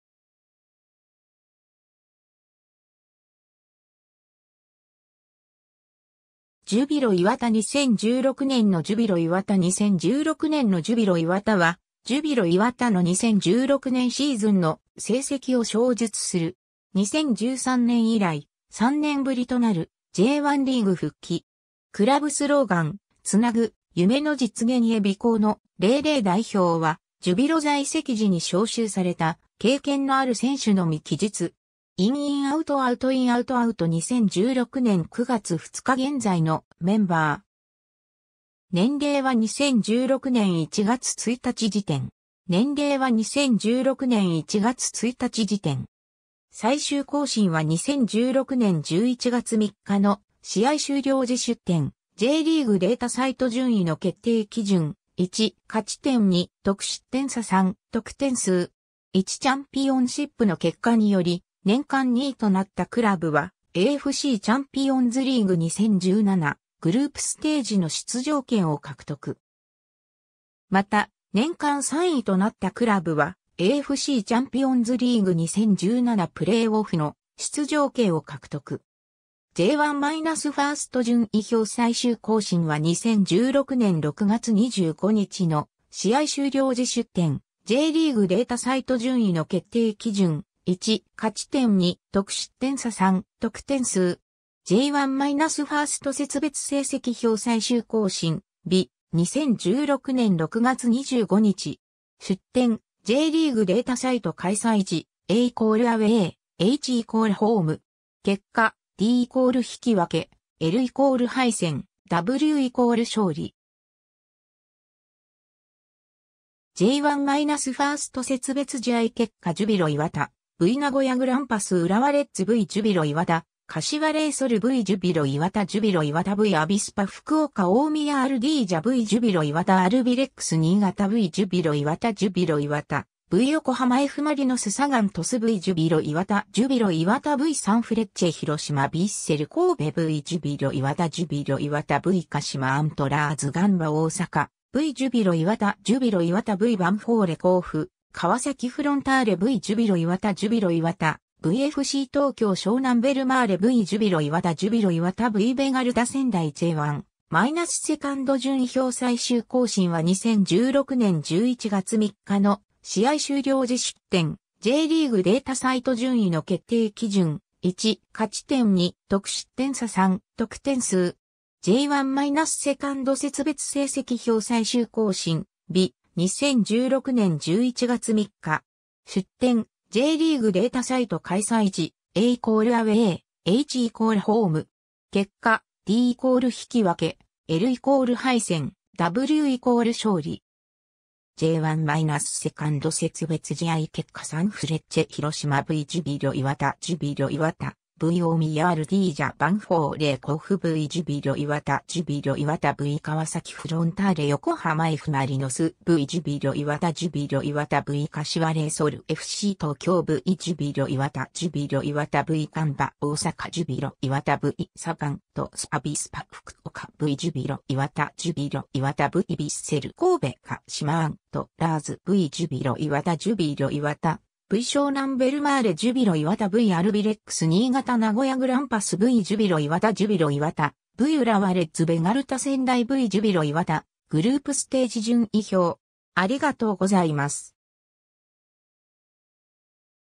2016年のジュビロ磐田
ジュビロ磐田 」 2016年のジュビロ磐田 2016年のジュビロ磐田は、ジュビロ磐田の2016年シーズンの成績を詳述する。
 2013年以来、3年ぶりとなるJ1リーグ復帰。
 クラブスローガン：繋ぐ 夢の実現へ ※ 備考の「(元)〇〇代表」は、ジュビロ在籍時に招集された経験のある選手のみ記述。
 IN IN (レンタル) OUT OUT (レンタル) IN OUT OUT (レンタル) 2016年9月2日現在のメンバー。
※ 年齢は2016年1月1日時点。
 最終更新は2016年11月3日の試合終了時 出典: J. League Data Site 順位の決定基準: 1. 勝点; 2. 得失点差; 3. 得点数. 1 チャンピオンシップの結果により年間2位となったクラブはAFCチャンピオンズリーグ2017 グループステージの出場権を獲得。
また、年間3位となったクラブはAFCチャンピオンズリーグ2017 プレーオフの出場権を獲得。
 J1-1st 順位表 最終更新は2016年6月25日の試合終了時 出典: J. League Data Site 順位の決定基準: 1. 勝点; 2. 得失点差; 3. 得点数. J1-1st 節別成績表 最終更新日：2016年6月25日. 出典：J. League Data Site 開催地：A = アウェー、H = ホーム。
結果： D = 引き分け、 L = 敗戦、W = 勝利。
 J1-1st 節別試合結果 ジュビロ磐田 v 名古屋グランパス 浦和レッズ v ジュビロ磐田 柏レイソル v ジュビロ磐田 ジュビロ磐田 v アビスパ福岡 大宮アルディージャ v ジュビロ磐田 アルビレックス新潟 v ジュビロ磐田 ジュビロ磐田 v 横浜F・マリノス サガン鳥栖 v ジュビロ磐田 ジュビロ磐田 v サンフレッチェ広島 ヴィッセル神戸 v ジュビロ磐田 ジュビロ磐田 v 鹿島アントラーズ ガンバ大阪 v ジュビロ磐田 ジュビロ磐田 v ヴァンフォーレ甲府 川崎フロンターレ v ジュビロ磐田 ジュビロ磐田 v FC東京 湘南ベルマーレ v ジュビロ磐田 ジュビロ磐田 v ベガルタ仙台 J1-2nd 順位表 最終更新は2016年11月3日の試合終了時 出典: J. League Data Site 順位の決定基準: 1. 勝点; 2. 得失点差; 3. 得点数. J1-2nd 節別成績表 最終更新日：2016年11月3日. 出典：J. League Data Site 開催地：A = アウェー、H = ホーム。
 J1-2nd 節別試合結果 サンフレッチェ広島 v ジュビロ磐田 ジュビロ磐田 v 大宮アルディージャ ヴァンフォーレ甲府 v ジュビロ磐田 ジュビロ磐田 v 川崎フロンターレ 横浜F・マリノス v ジュビロ磐田 ジュビロ磐田 v 柏レイソル FC東京 v ジュビロ磐田 ジュビロ磐田 v ガンバ大阪 ジュビロ磐田 v サガン鳥栖 アビスパ福岡 v ジュビロ磐田 ジュビロ磐田 v ヴィッセル神戸 鹿島アントラーズ v ジュビロ磐田 ジュビロ磐田 v 湘南ベルマーレ ジュビロ磐田 v アルビレックス新潟 名古屋グランパス v ジュビロ磐田 ジュビロ磐田 v 浦和レッズ ベガルタ仙台 v ジュビロ磐田 グループステージ 順位表